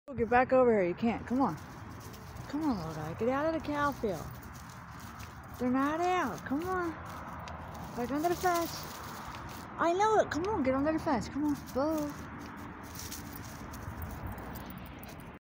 Under electrified fence at Farm/Try to coax him out
My boy Boo goes under the electrified fence to look for his "Cow friends". I had to coax him to come back under! Didn't want a shocking experience! He 💚s 🐄s